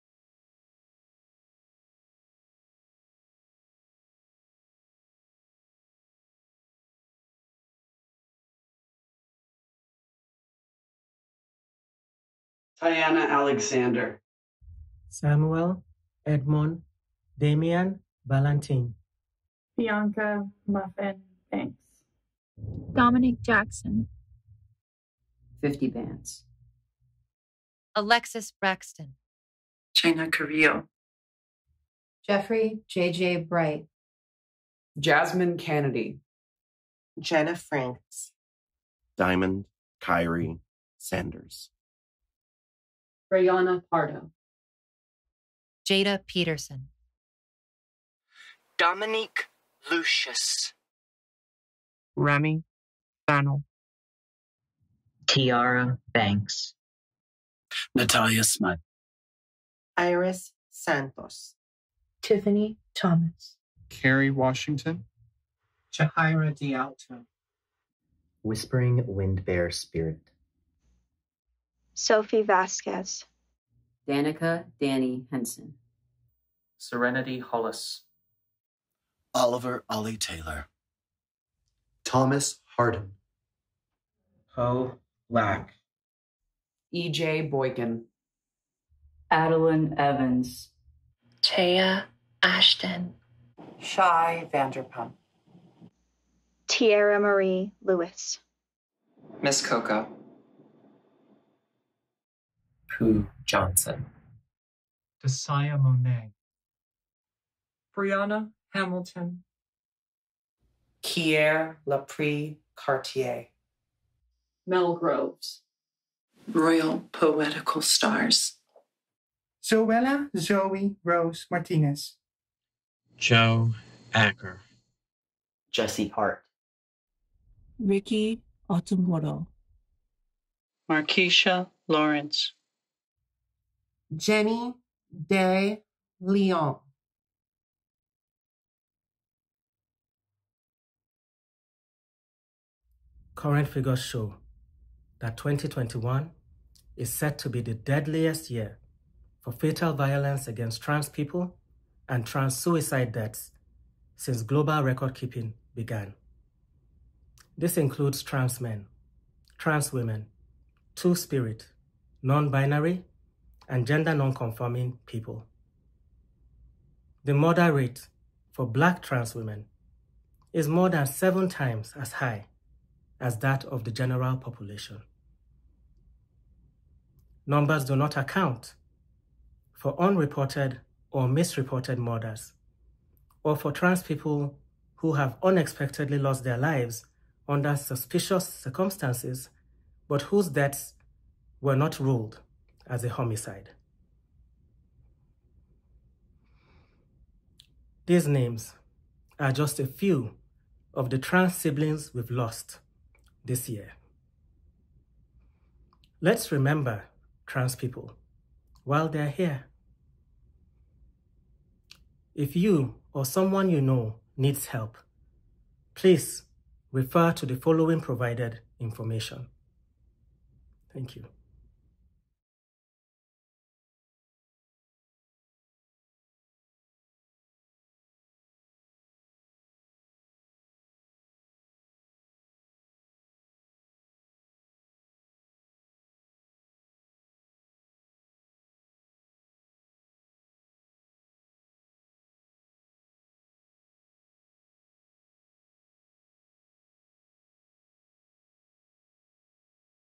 Transgender Day of Remembrance 2021
// CW: violence against trans and gender non-conforming people

It is not enough to be aware when so many lights in our community are extinguished for the sole crime of shining. On this Transgender Day Of Remembrance, in the deadliest year for trans people on record, we remember and honor all of our trans and nonbinary siblings who have been lost to violence this year.

Tyianna Alexander
Samuel Edmund Damián Valentín
Bianca “Muffin” Bankz
Dominique Jackson
Fifty Bandz
Alexus Braxton
Chyna Carrillo
Jeffrey “JJ” Bright
Jasmine Cannady
Jenna Franks
Diamond Kyree Sanders
Rayanna Pardo
Jaida Peterson
Dominique Lucious
Remy Fennell
Tiara Banks
Natalia Smut
Iris Santos
Tiffany Thomas
Keri Washington
Jahaira DeAlto
Whispering Wind Bear Spirit
Sophie Vásquez
Danika “Danny” Henson
Serenity Hollis
Oliver “Ollie” Taylor
Thomas Hardin
Poe Black
EJ Boykin
Aidelen Evans
Taya Ashton
Shai Vanderpump
Tierramarie Lewis
Miss CoCo
Pooh Johnson
Disaya Monaee
Briana Hamilton
Kiér Laprí Kartier
Mel Groves
Royal Poetical Starz
Zoella “Zoey” Rose Martinez
Jo Acker
Jessi Hart
Rikkey Outumuro
Marquiisha Lawrence
Jenny De Leon

We remember. We say their names.